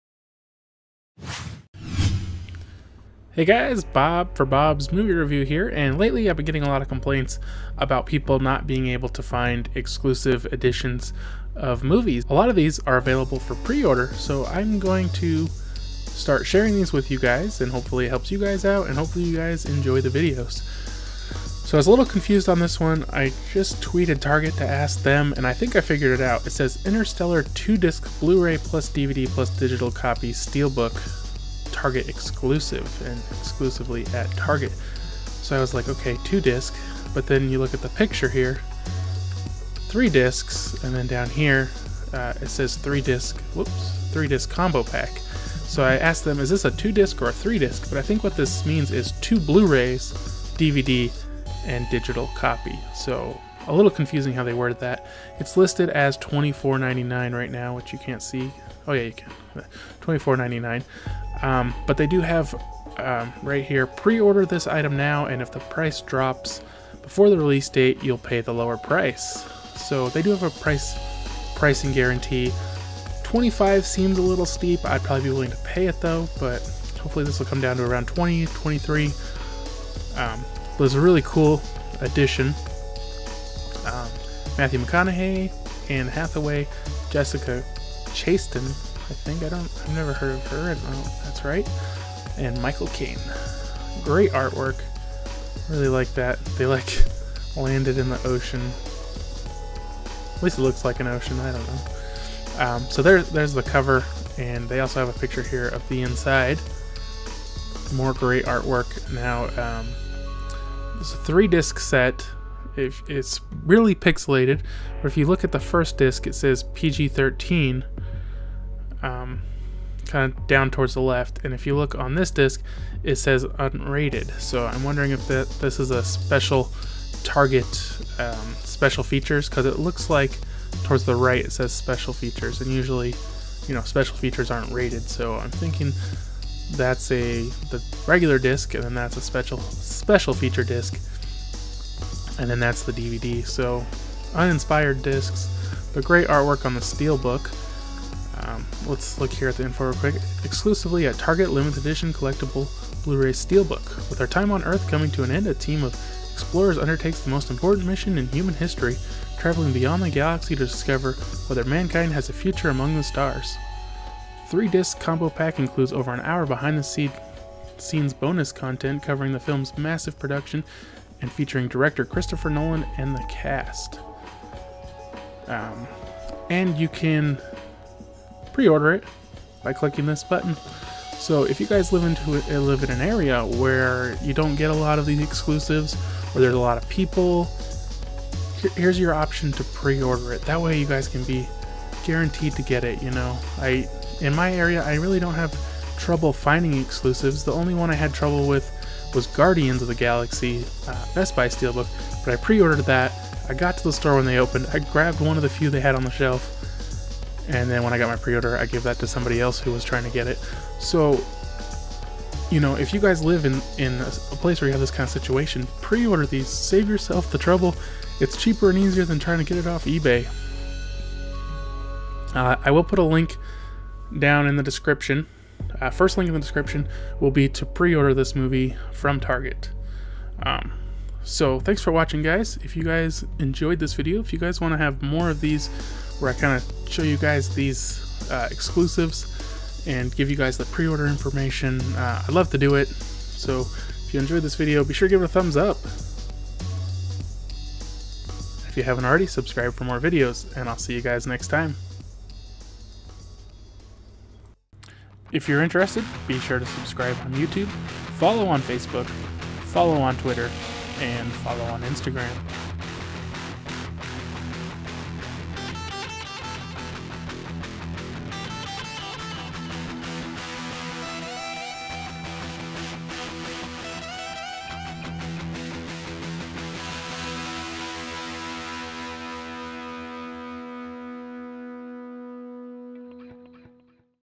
Interstellar Target Exclusive Steelbook Blu-Ray Preorder
Discussion of the Interstellar Target Exclusive Steelbook Preorder

- Interstellar
Rated PG-13 , Runtime 169 Minutes
Rating 8.8 / 10 , Bob's Rating N / A
Director Christopher Nolan
Stars Ellen Burstyn , Matthew McConaughey , Mackenzie Foy ,
John Lithgow , Timothee Chalamet , David Oyelowo , Collette Wolfe ,
Francis X. McCarthy , Bill Irwin , Andrew Borba , Wes Bentley ,
William Devane , Michael Caine , David Gyasi

Interstellar Preorder
Interstellar Pre Order
Interstellar Pre-Order
Target Exclusive Steelbook